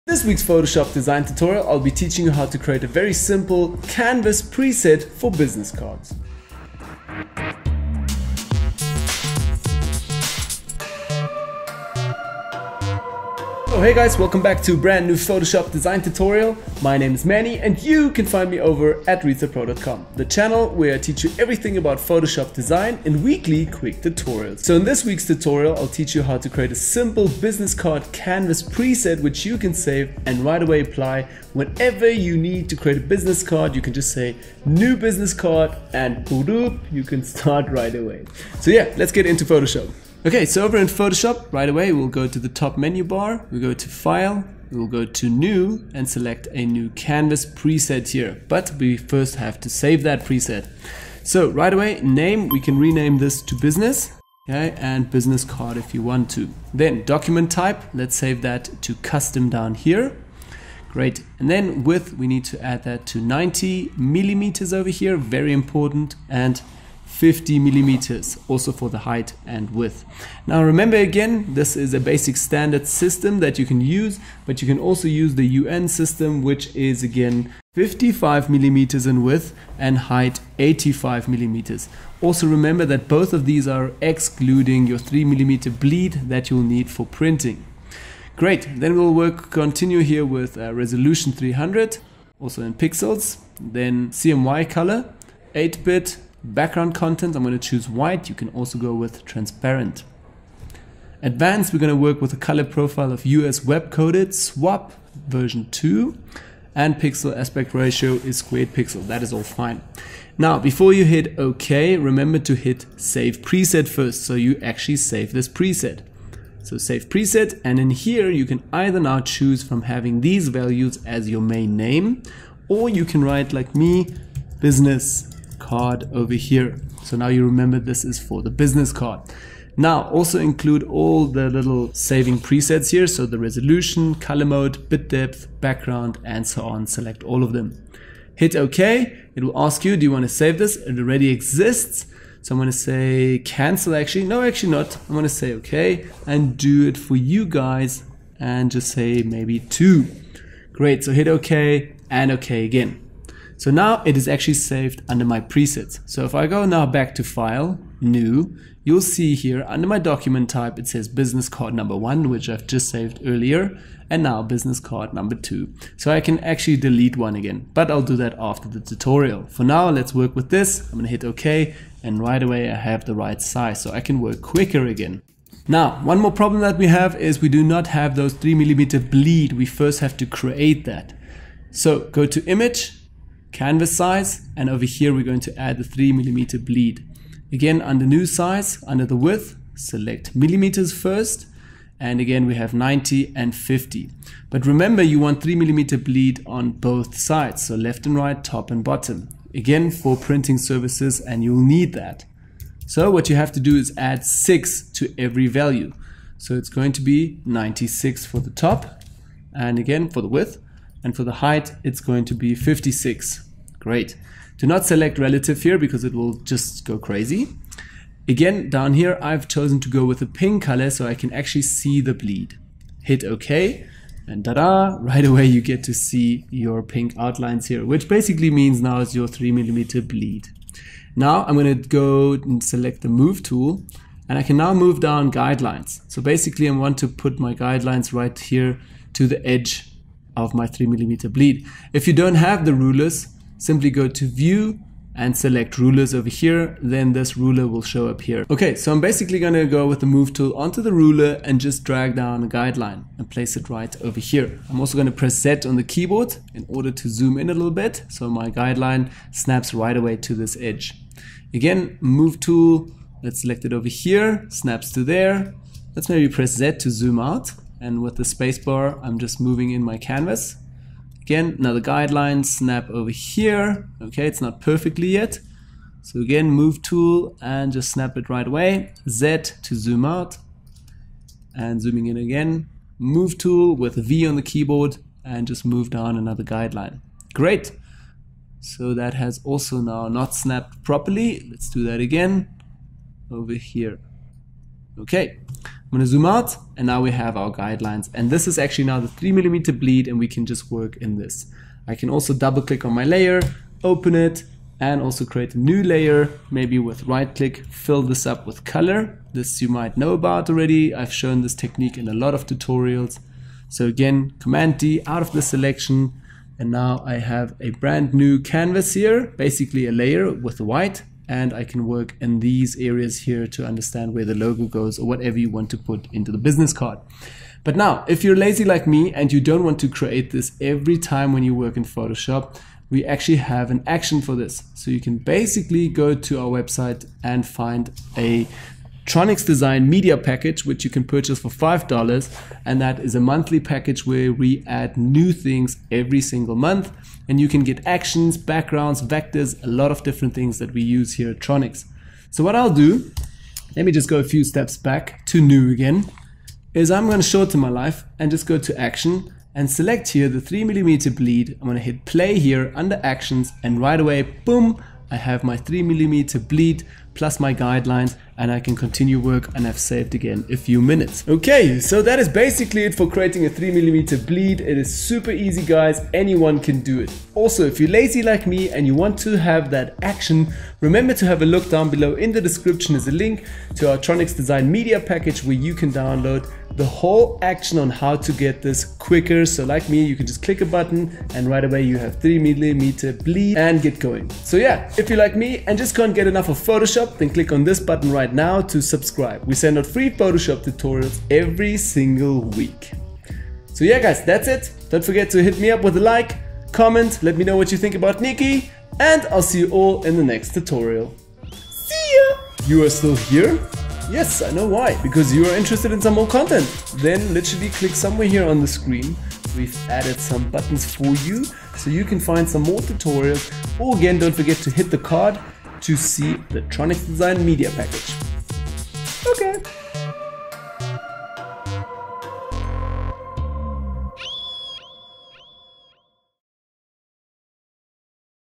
How To Save A Business Card Canvas Preset In Photoshop
About this week's episode:

Today we're looking into how to set up Photoshop presets. To be more specific how to set up a business card canvas size preset. This is necessary or can be a good help, especially if you design buinsees cards on a regular bases. The aim here is to save you time when it comes to your photoshop design workflow.